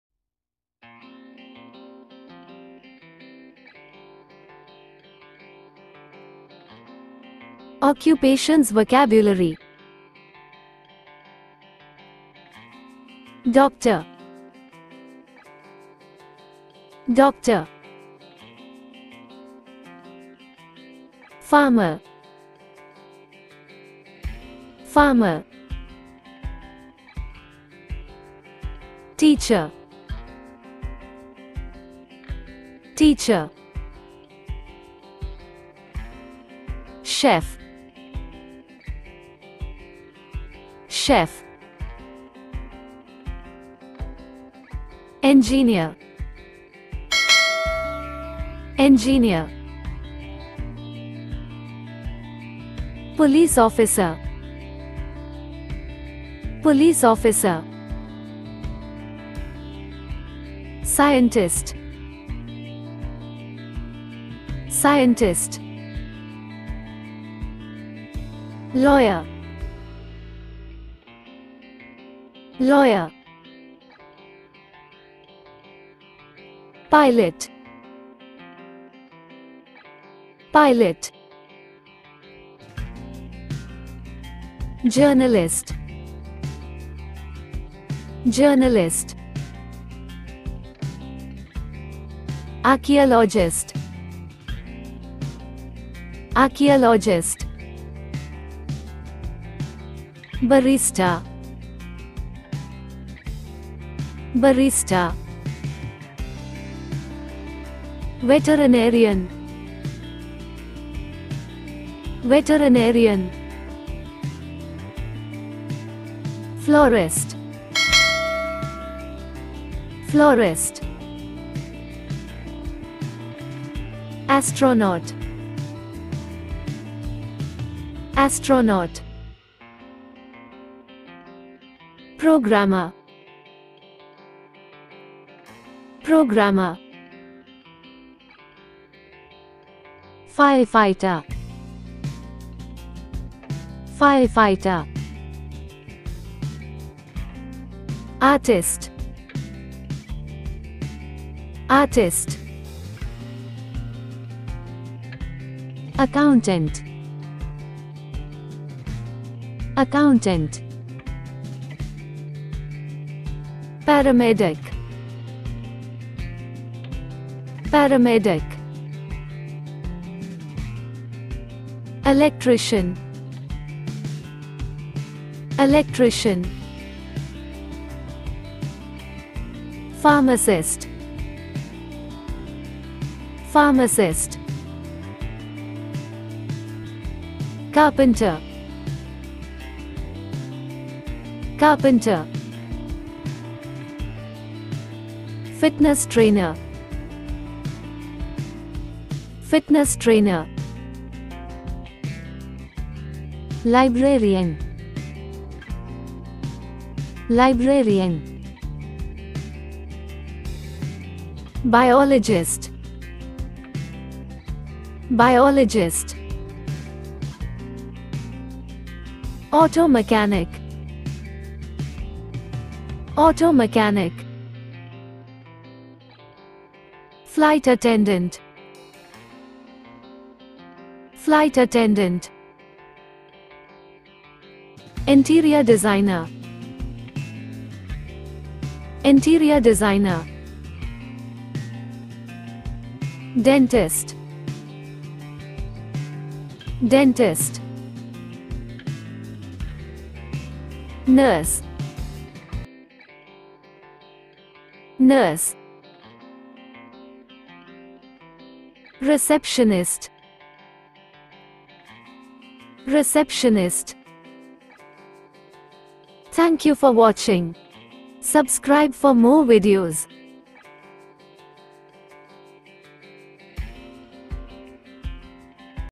Unlock Your Career Lexicon 🌟: Explore Illustrated Occupation & Jobs Names with Vibrant Pictures 🎨
Immerse yourself in the beauty of language as we bring occupations to life through vivid illustrations. 🌐 This isn't just a vocabulary lesson; it's a visual adventure designed to make learning enjoyable and memorable. 🗣️✨

Don't miss out on the chance to enrich your lexicon in a fun and engaging way. 🌈🔍

👉 Comment your favorite occupation 🗨️